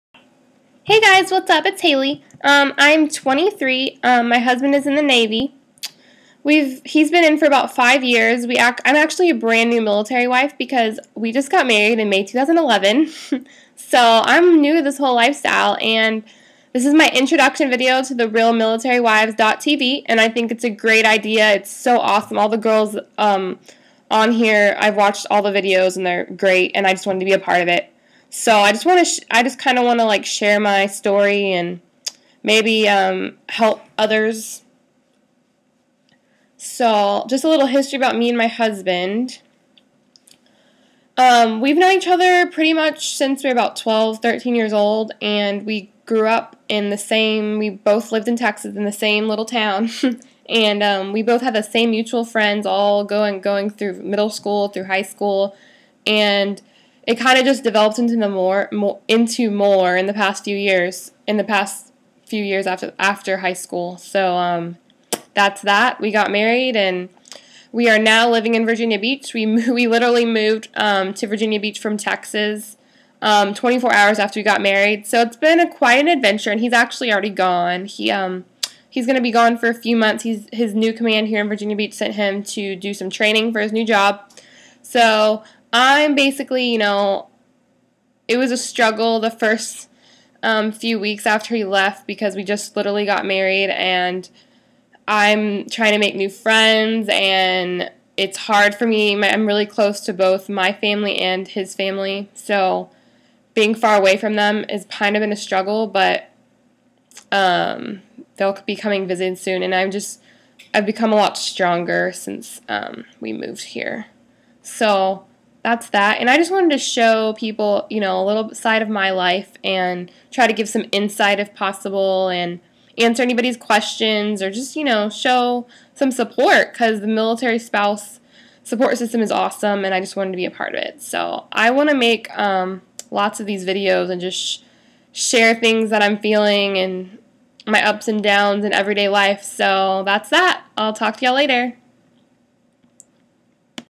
Navy wife intro to RealMilitaryWives.TV
Meet Haley she is a brand new navy wife.